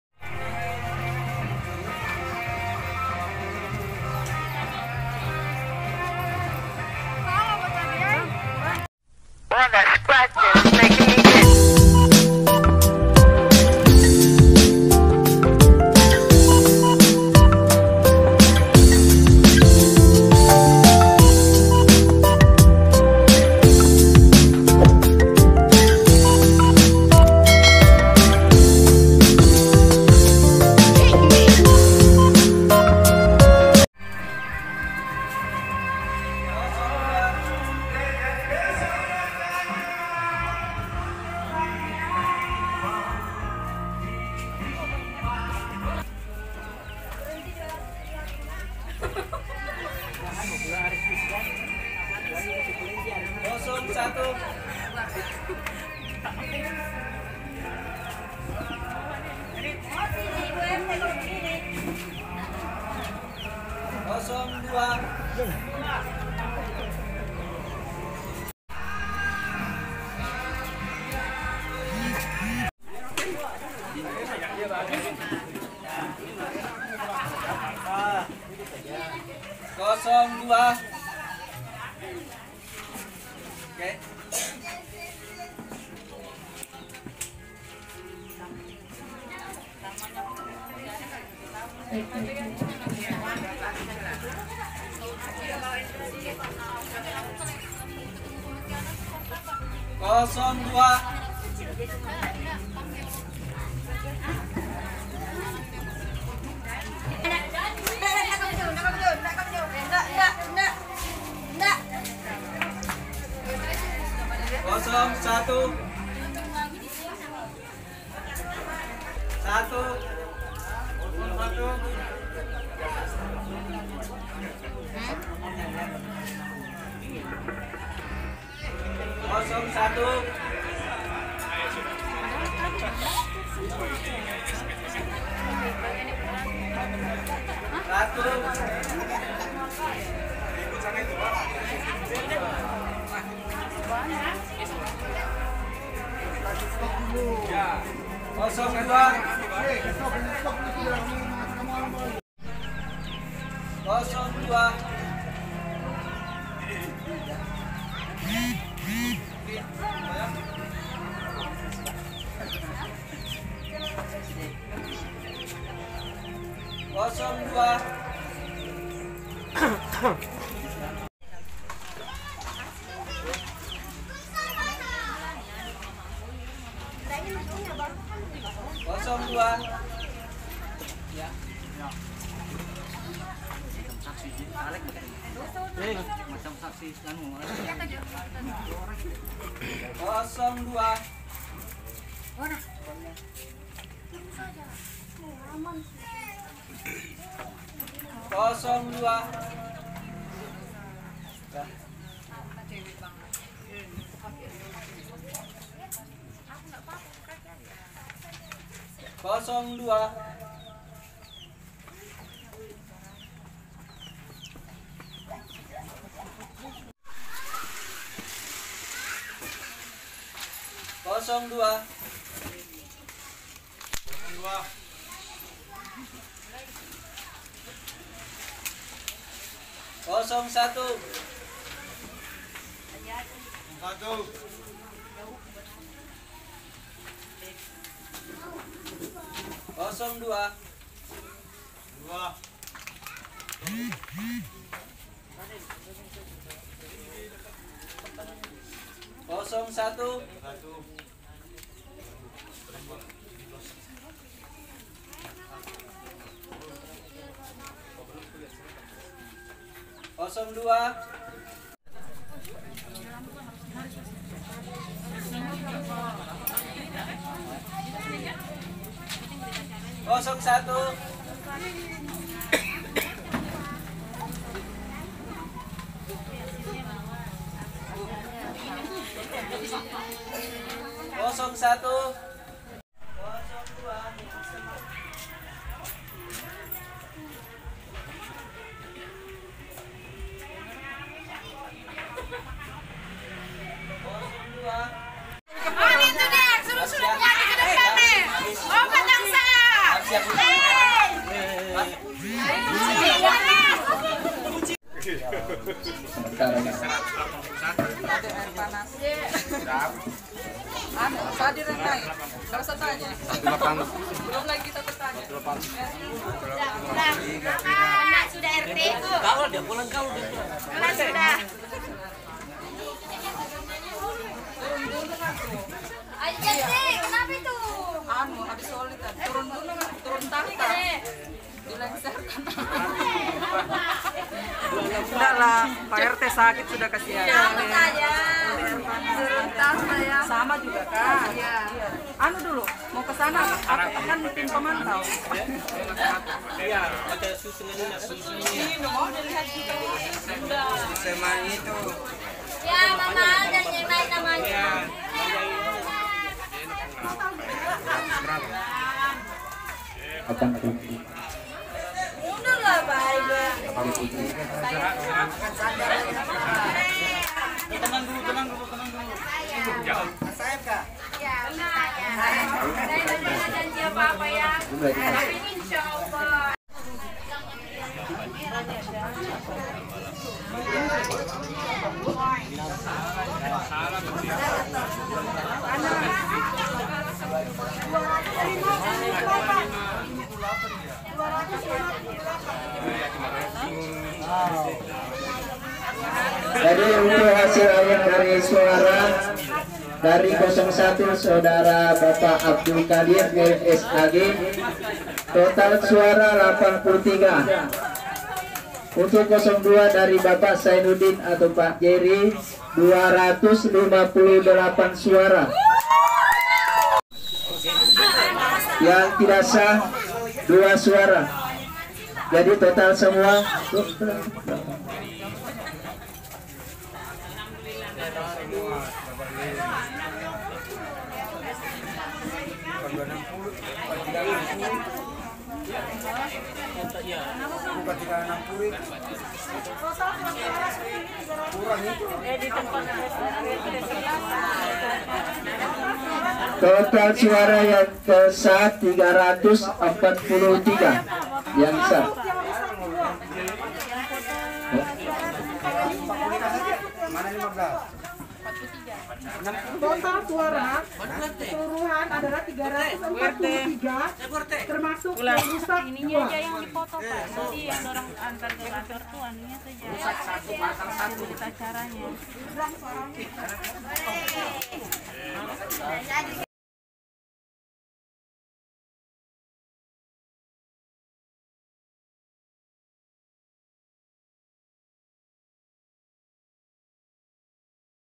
PEMILIHAN KETUA RT NUNUKAN TIMUR, PETAHANA MENANG MUTLAK
Pemilihan ketua RT di Kelurahan Nunukan Timur, Kabupaten Nunukan digelar serentak pada Sabtu-Minggu, 26-27 Juni 2021.

Pemilihan Ketua RT 20 dimenangkan oleh calon petahana Zainuddin "Jerry" dengan perolehan 258 suara dan penantangnya Abdul Kadir L, SAg mendapatkan 83 suara dan dua surat suara tidak sah.

Sebanyak 343 warga dari 410 orang yang tercatat dan memiliki hak suara. Pada saat pemilihan, warga wajib pilih membawa surat pemberitahuan dan KTP-nya untuk diperlihatkan kepada panitia pemilihan.

Sebanyak 21 RT di Kelurahan Nunukan Timur yang menggelar pemilihan ketua secara serentak.

Kunjungi terus dan tonton BERANDATIMUR Channel's yang menyajikan berita-berita terbaru dari wilayah perbatasan Indonesia-Malaysia di Kabupaten Nunukan.